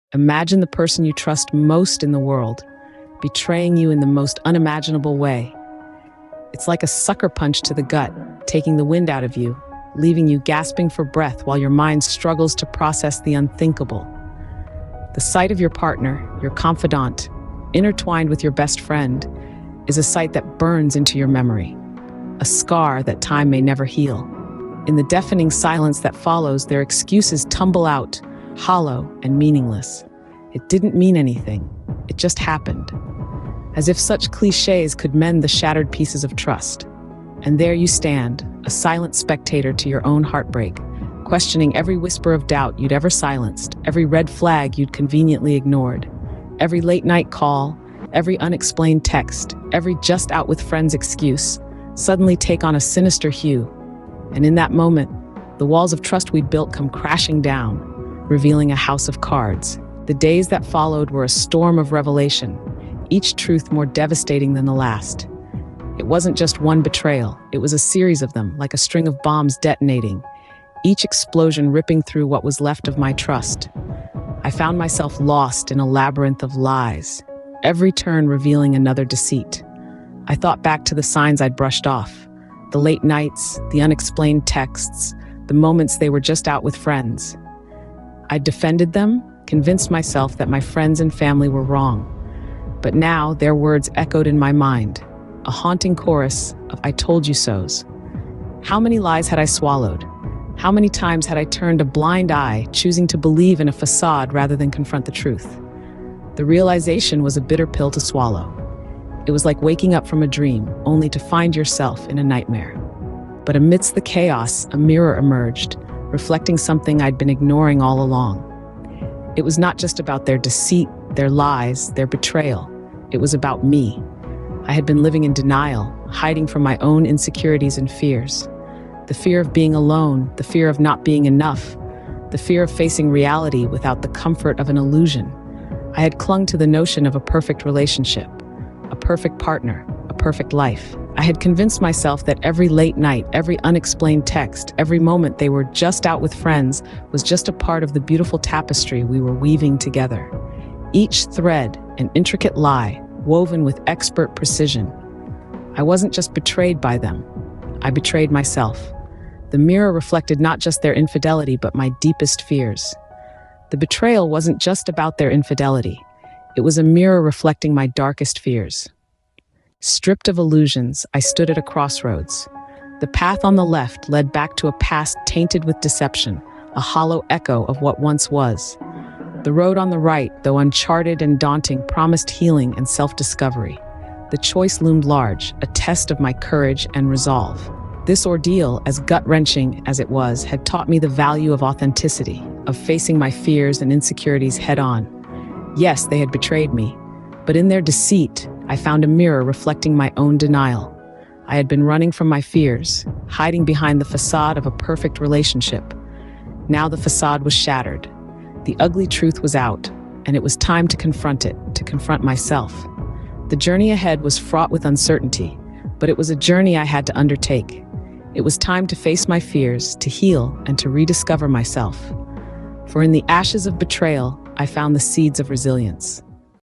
Betrayal at Heart: Unraveling Love's Greatest Deceit
My world shattered when I walked into our bedroom and found my partner with my best friend. This video uncovers the painful moment of betrayal that felt like a nuclear explosion in the heart of my trust. I share the raw emotions and the aftermath of demanding explanations that only led to more pain. Discover how I navigated through the flood of clichés and excuses, and the harsh truths that emerged about past infidelities. This isn't just a story of betrayal; it's a journey into self-awareness, confronting my own fears and insecurities that had allowed me to ignore the signs. Join me as I explore the process of dismantling the illusions I held about love and the challenging yet necessary path toward healing and embracing my own truths.